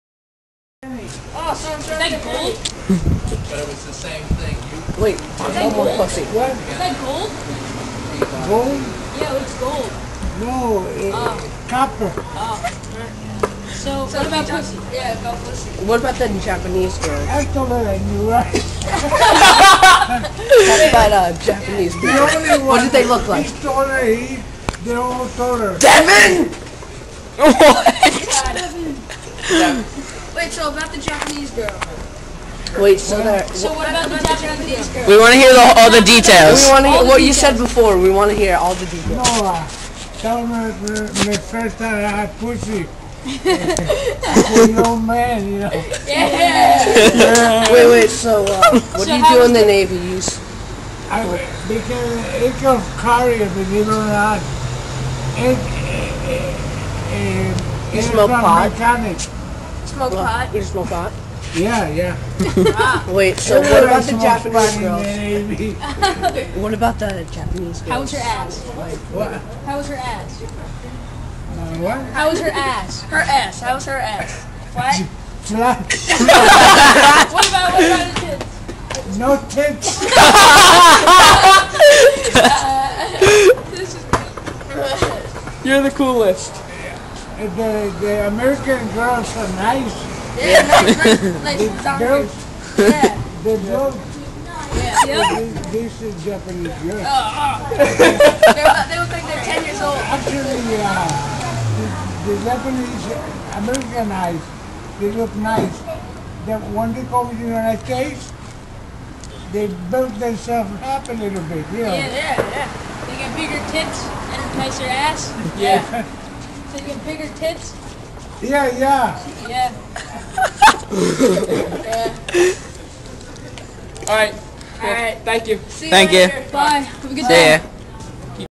Homeless guy at shop right
so we were at shop right messing around and we met this guy he started to talk to us and we couldnt help but laugh then he started to talk about how he did pot and how he fucked a Japanese girl hes insane he picks used cigarettes off the ground but hes a cool guy please rate comment and subscribe